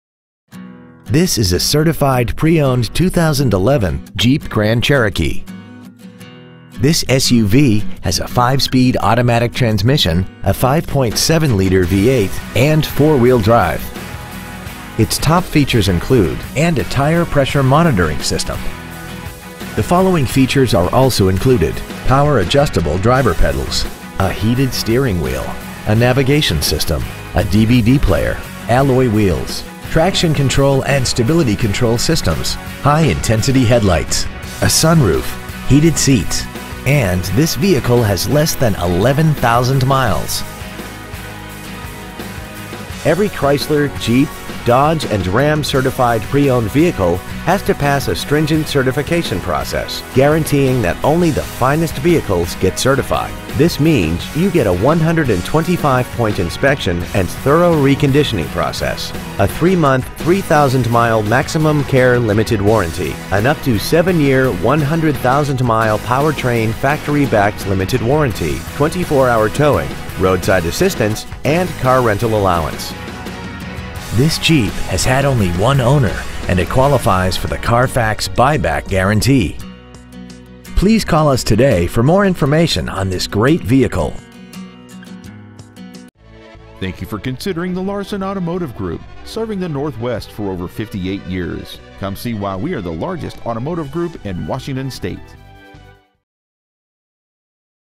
2011 Jeep Grand Cherokee Puyallup WA
Year : 2011
 Make : Jeep
 Model : Grand Cherokee
 Engine : 5.7L V8 OHV 16V
 Trans . : 5 Spd Automatic
 Exterior : White
 Miles : 10,780
 Interior : Black
 Stock : 9090

 2011 Jeep Grand Cherokee Puyallup WA